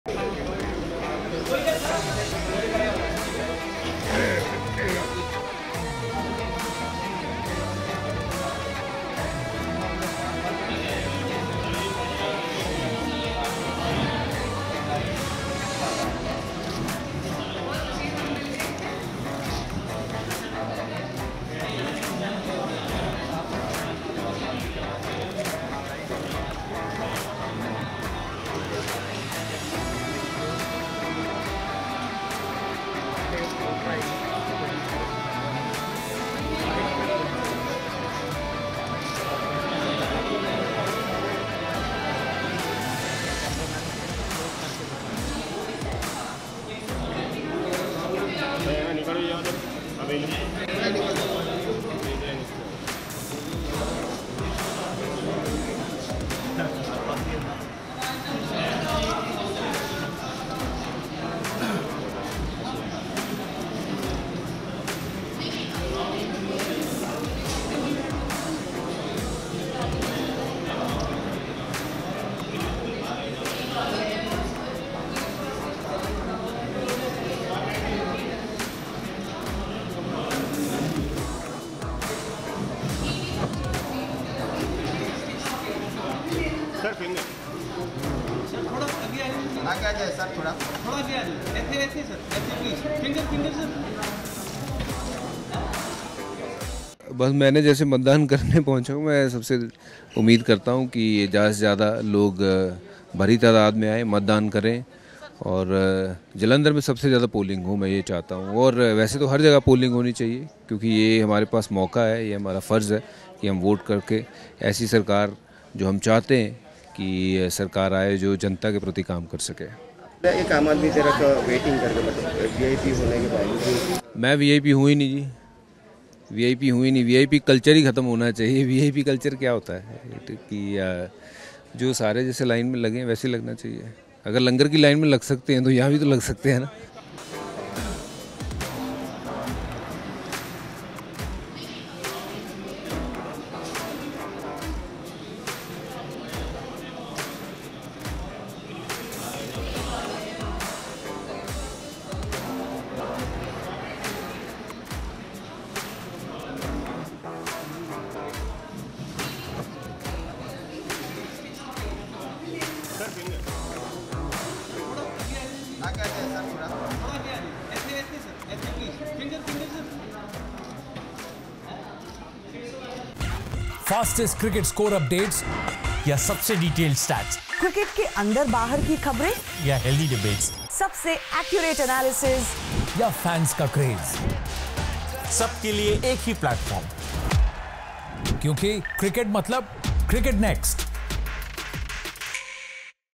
Harbhajan Singh Casts Vote In Final Phase Of Lok Sabha Elections 2024
Former India spinner and AAP Rajya Sabha MP Harbhajan Singh cast his vote in the last and final phase of Lok Sabha Elections 2024 on Saturday.